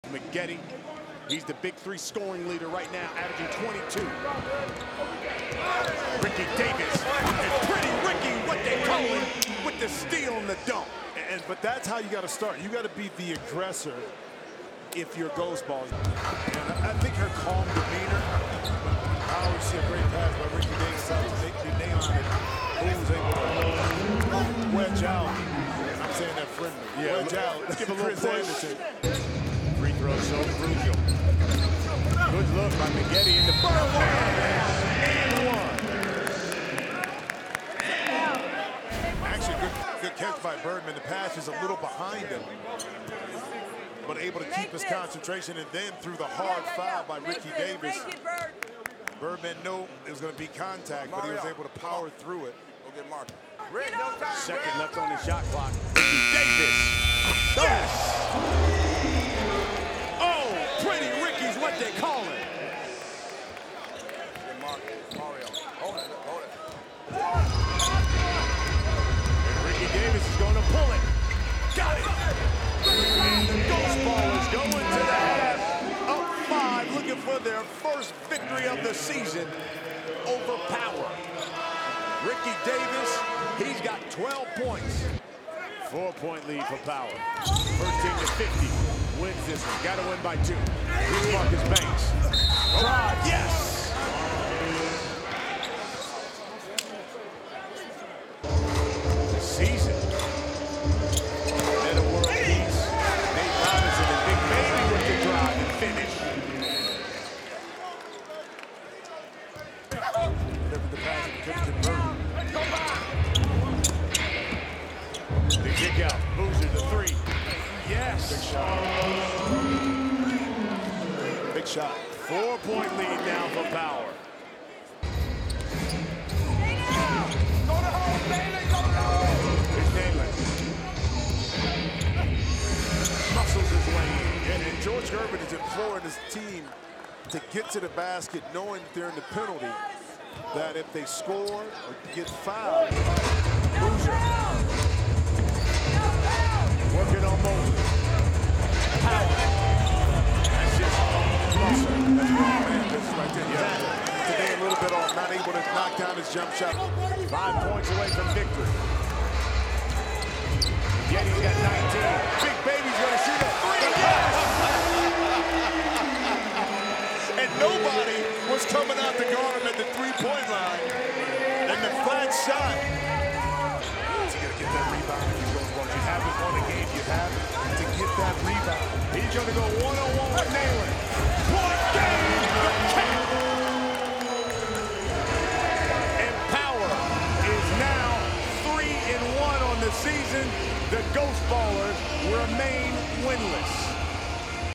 Ghost Ballers vs Power | BIG3 HIGHLIGHTS
Glen 'Big Baby' Davis helps lead Power past Ghost Ballers 50-45 to improve to 3-1.